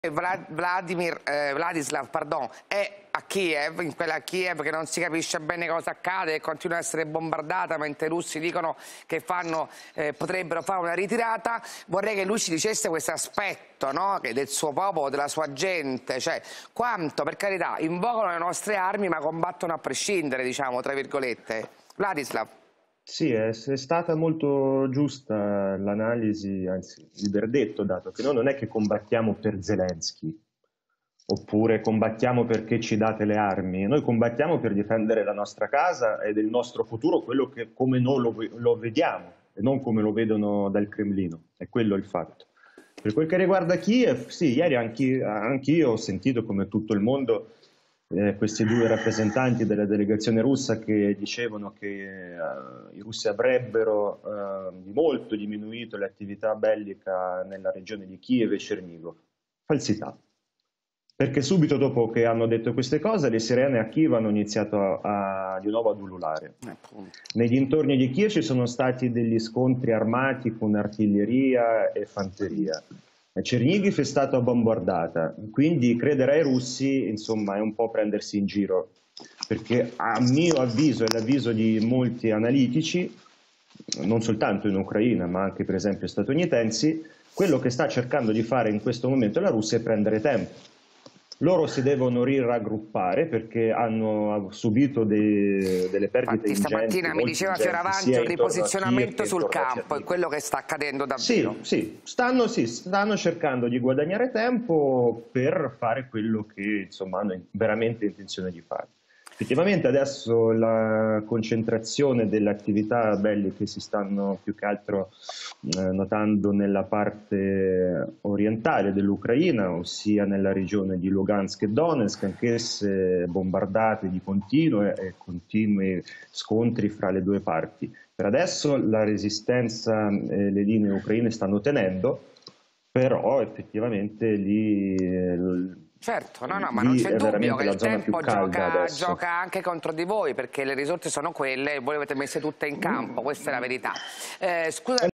Il giornalista ucraino Vladislav Maistrouk: "Credere ai russi è un po' prendersi in giro, ...
Il giornalista ucraino Vladislav Maistrouk: "Noi non combattiamo per Zelensky o perché ci date le armi, noi combattiamo per difendere la nostra casa e il nostro futuro, per come lo vediamo noi e non per come lo vedono dal Cremlino".